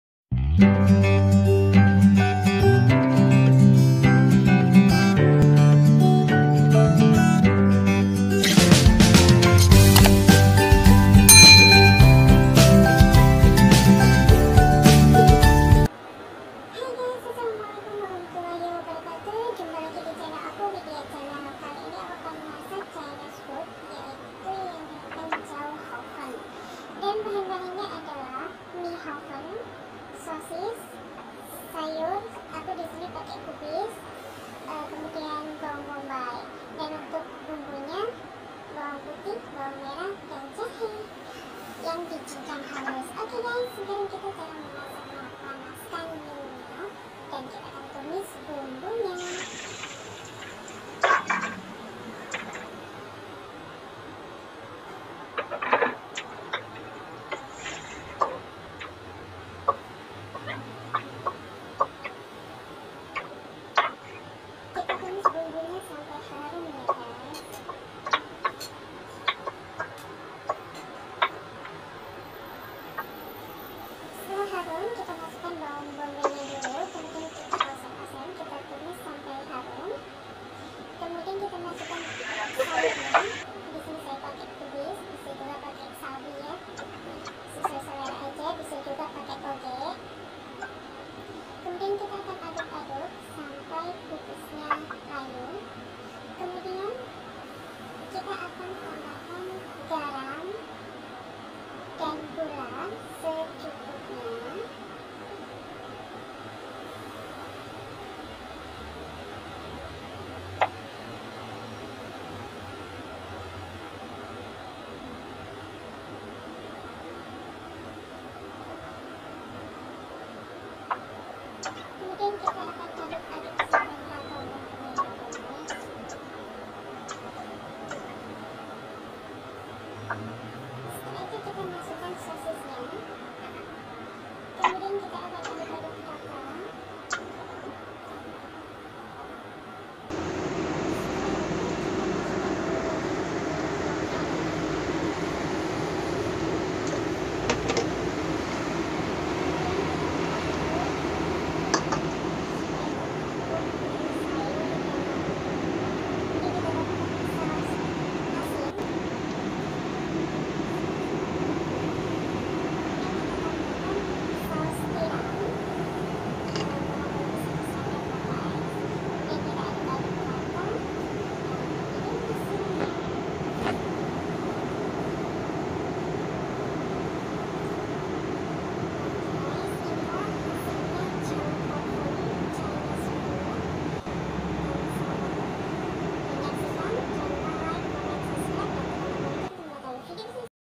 JAU HOUFEN CHAINISE FOOD#widyawati chanel#
bahan:
              mie houfen
              1 bawang bombay
              4biji sosis
               sayur secukup nya
bumbu:
               4 siung bawang putih
                2siung bawang merah
                      cincang halus
                2 lembar jahe